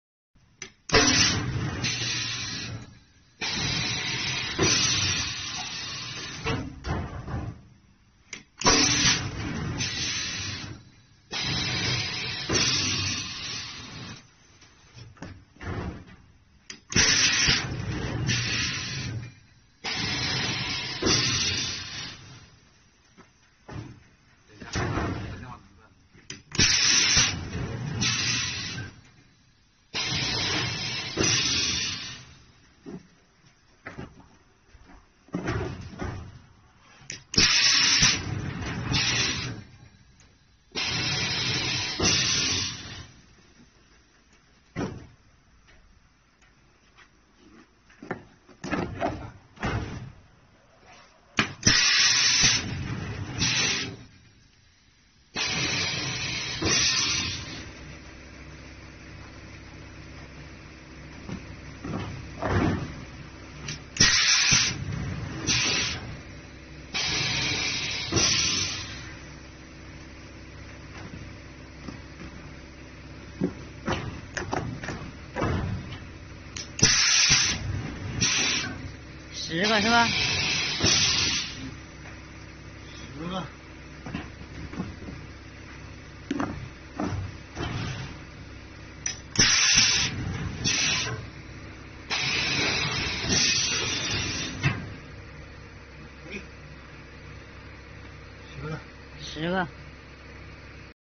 How Henan Mingjie Tyre pretreat equipment of Tyre Compacting Machine working
This machine can compact the tyre after cutting the sidewall, so utilization of pyrolysis plant reactor volume could be greatly improved.